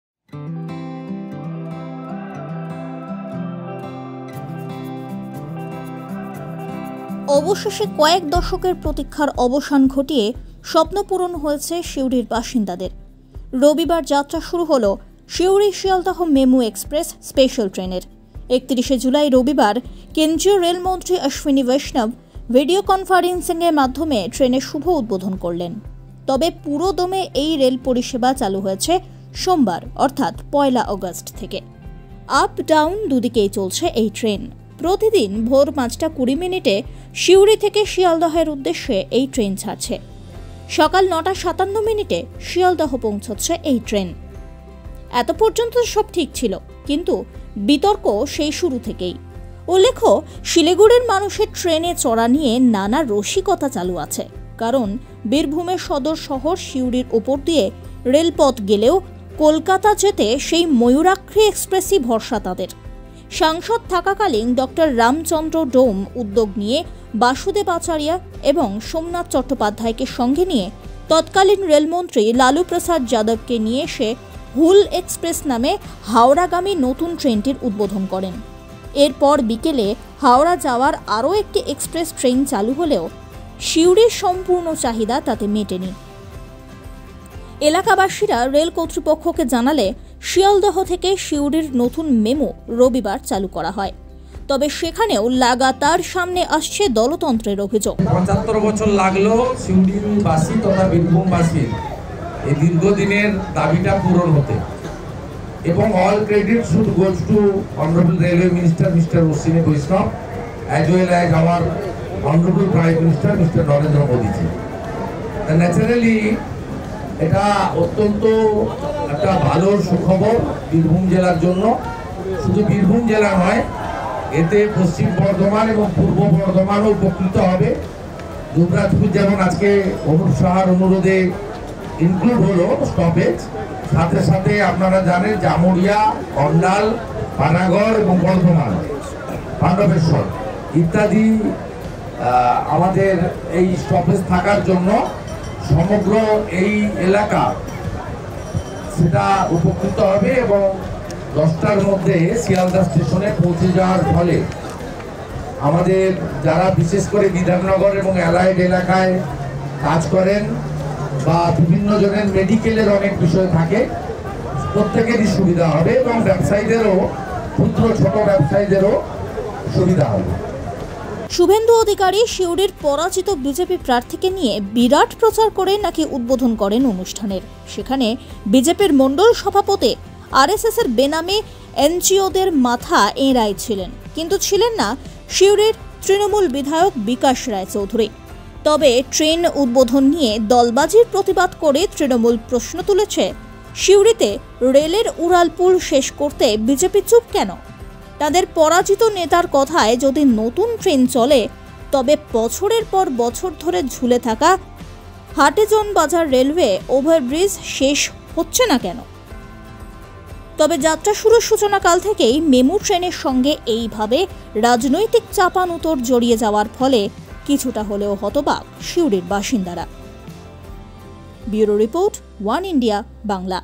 শুরু থেকেই বিতর্কে মেমু, ট্রেন কোন দলের? প্রশ্নে উত্তাল রাজনৈতিক মহল |OneIndia Bnegali
শুরু থেকেই বিতর্কে মেমু, ট্রেন কোন দলের? প্রশ্নে উত্তাল রাজনৈতিক মহল